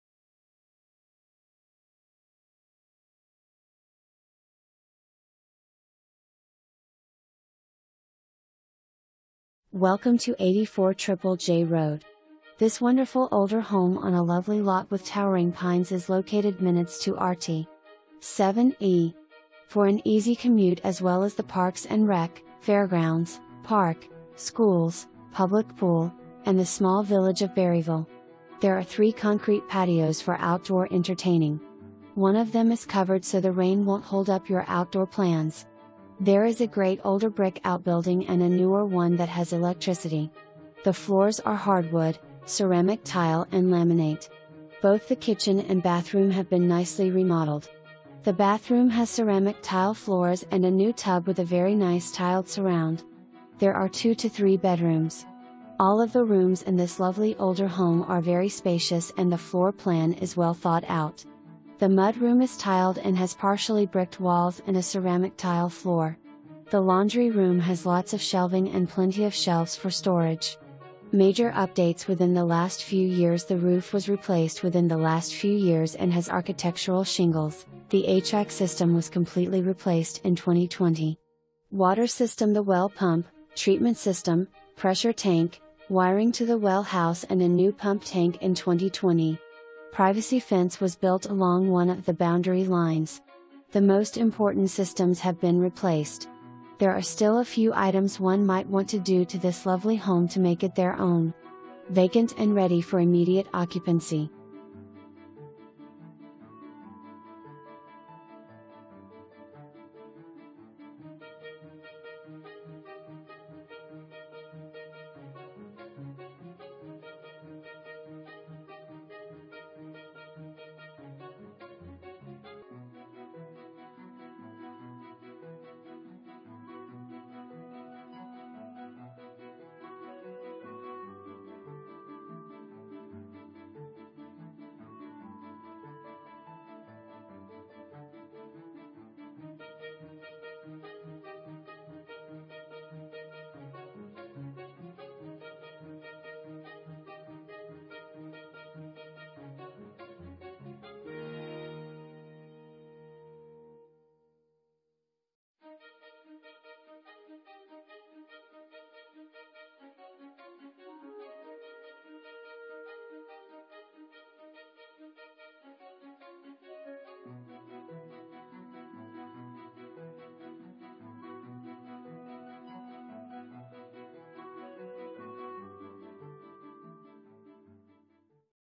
84 Triple J Road
84 Triple J Road
Berryville, VA 22611
$264,900, 3 bed, 1.00 bath, 1,176 SF, MLS# VACL112102

Welcome to 84 Triple J. Road. This Wonderful older home on a lovely lot with towering pines is located minutes to Rt. 7E. for an easy commute as well as the parks and rec., fairgrounds, park, schools, public pool, and the small village of Berryville. There are 3 concrete patios for outdoor entertaining. One of them is covered so the rain won't hold up your outdoor plans. There is a great older brick outbuilding and a newer one that has electricity. The floors are hardwood, ceramic tile and laminate. Both the kitchen and bathroom have been nicely remodeled. The bathroom has ceramic tile floors and a new tub with a very nice tiled surround. There are 2-3 bedrooms. All of the rooms in this lovely older home are very spacious and the floor plan is well thought out. The mud room is tiled and has partially bricked walls and a ceramic tile floor. The laundry room has lots of shelving and plenty of shelves for storage. MAJOR UP-DATES WITHIN THE LAST FEW YEARS- The roof was replaced within the last few years & has architectural shingles, the HVAC System was completely replaced in 2020. Water System- The well pump, treatment system, pressure tank, wiring to the well house and a new pump tank in 2020. Privacy Fence- Was built along one of the boundary lines. The most important systems have been replaced. There are still a few items one might want to do to this lovely home to make it their own. Vacant and ready for immediate occupancy.

1 full bath

Lisette Turner
CENTURY 21 Redwood Realty